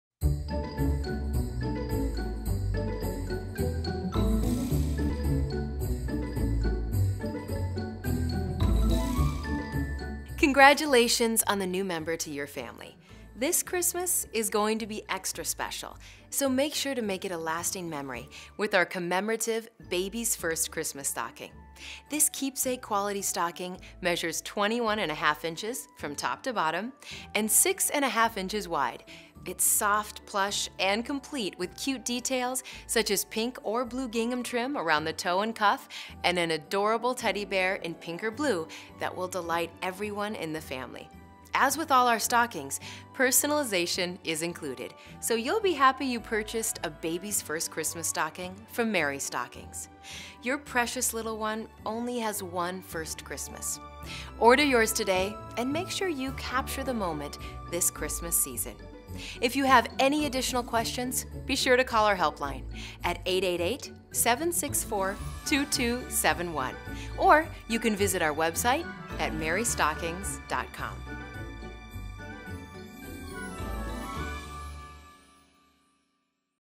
Baby's First Christmas Stocking | Boy or Girl
A Personalized stocking would be a great way to commemorate the 1st Christmas of the new addition to your family. Available for boys or girls, both stockings are complete with cute details you'll sure to cherish. If you desire, we can even embroider the year on the stocking for you. Just ask us!